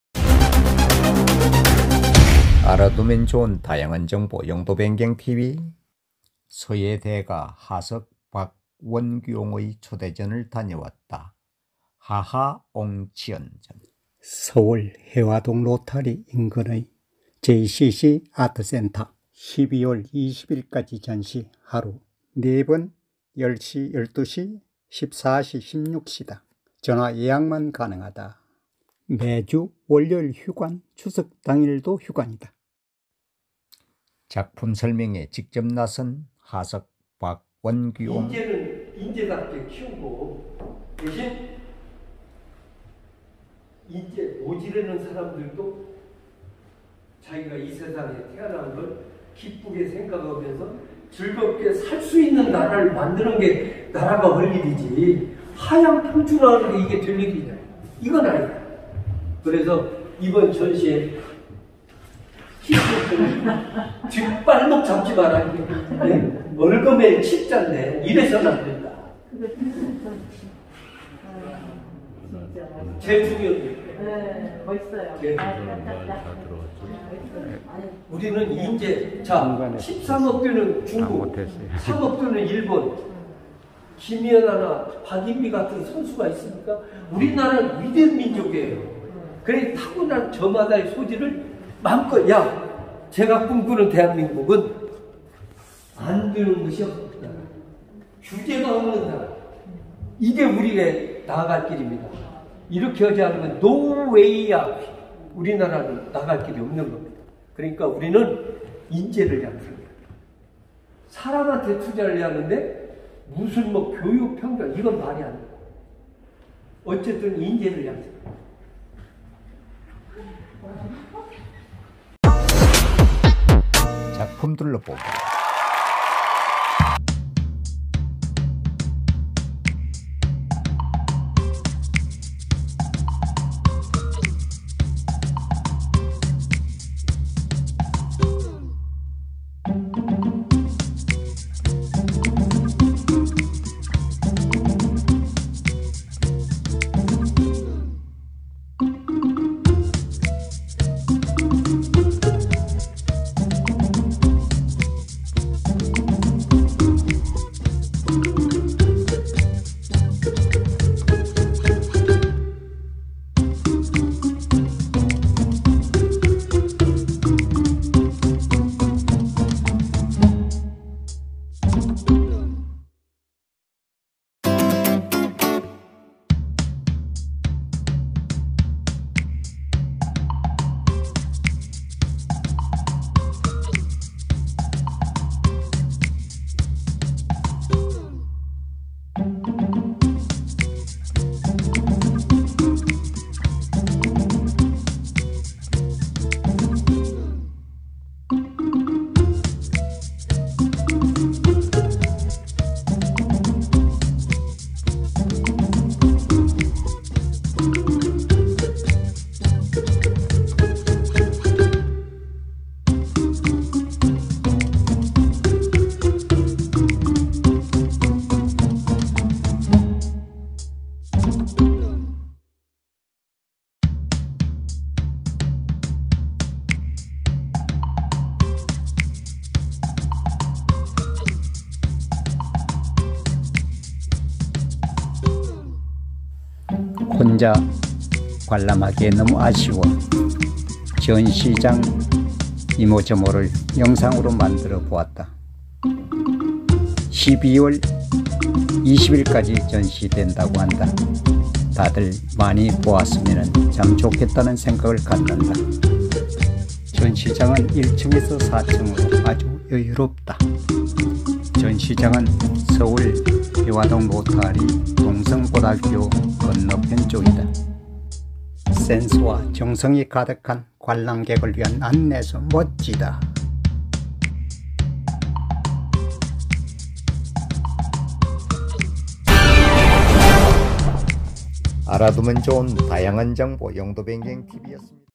서예전시 "하하옹치언" 하석 박원규 서예대가 개인초대전
JCC아트센터 서예대가 하석 박원규 개인초대전 "하하옹치언".2020,9,16~12,20, 하루 4회 10시 12시 14시 16시, 전화예약만가능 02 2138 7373. 매주월요일,추석당일 휴관